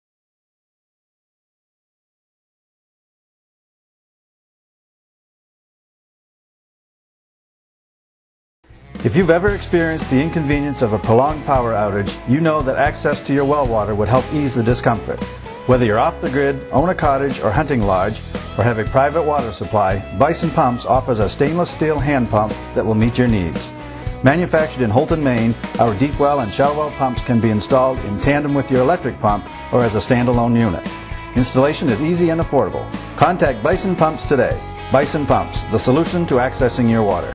Bison Pumps Commercial
A spot for Bison Pumps, makers of a non-electric hand water pump. More at BisonPumps.com.